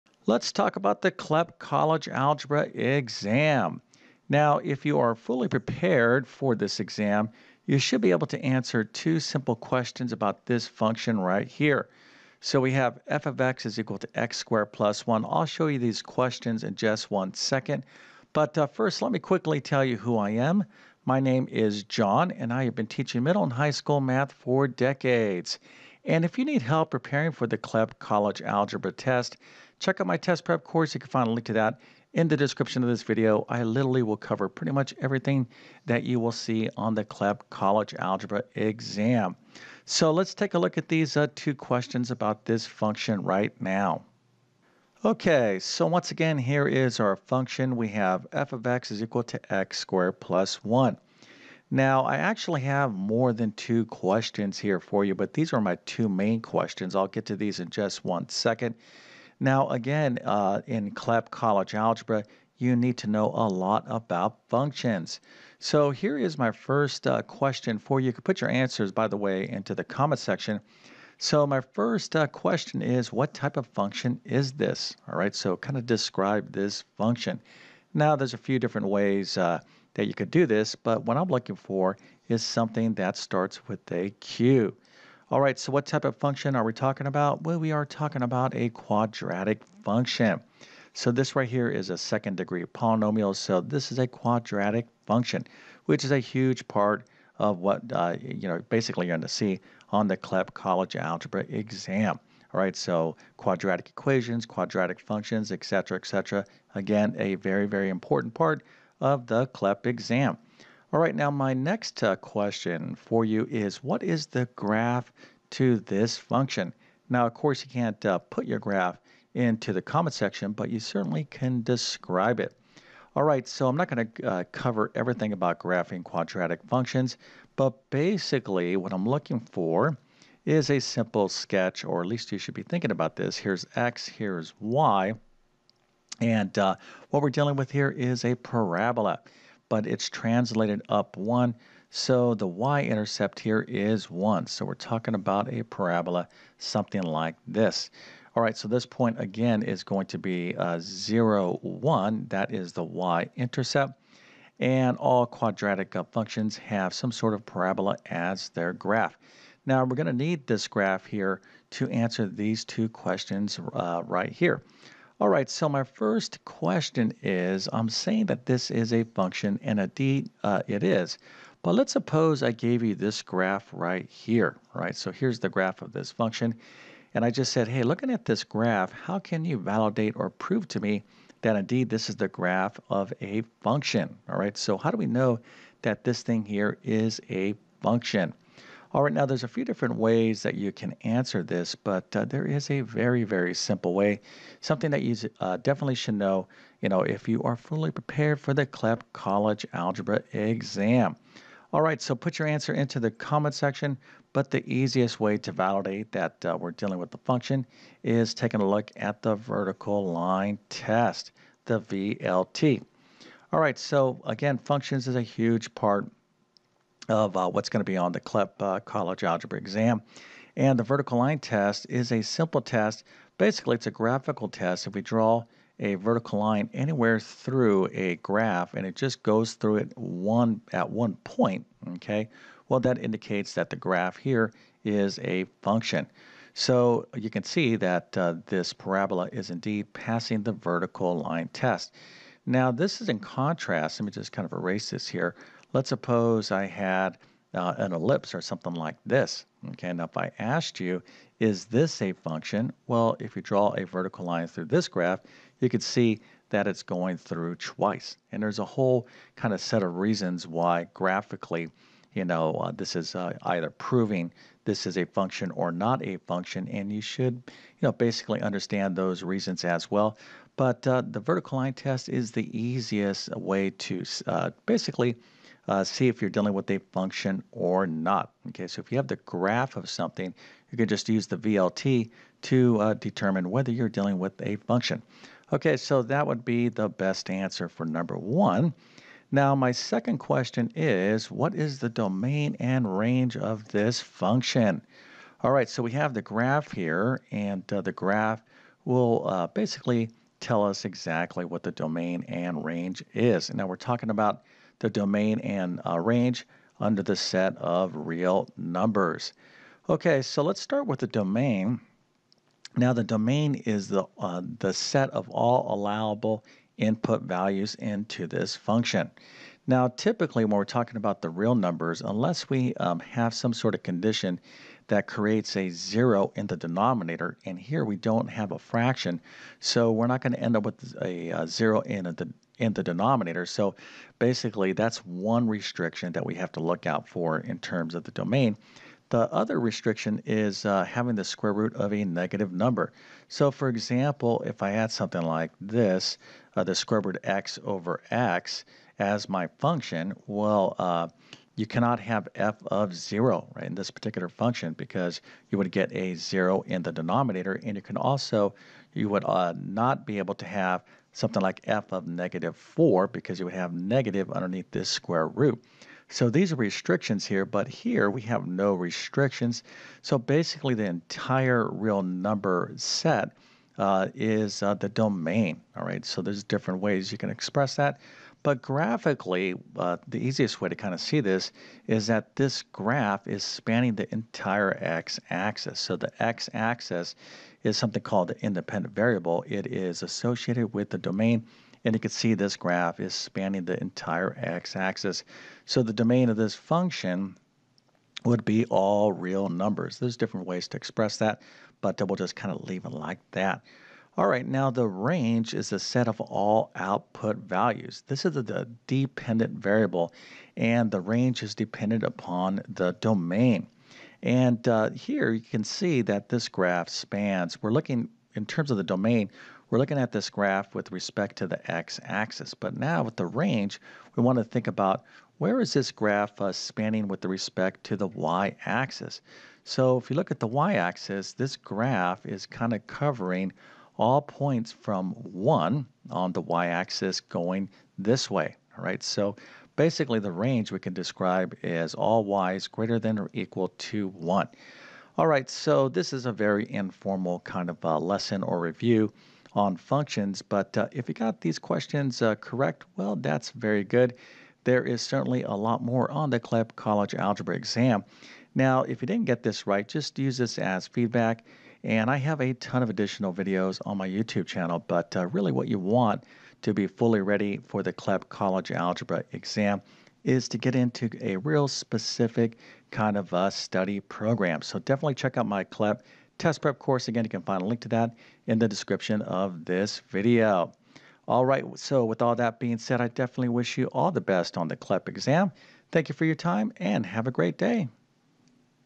CLEP College Algebra 2025 – Practice Problem
Pass the CLEP College Algebra Exam in 2025 and you will have taken advantage of a great opportunity to earn college credit. In this video I will go over a practice problem on functions, domain and range - a math skill you will need to know to pass the CLEP College Algebra exam. If you need math help in passing the CLEP College Algebra Exam check out my CLEP College Algebra Math Test Prep Course: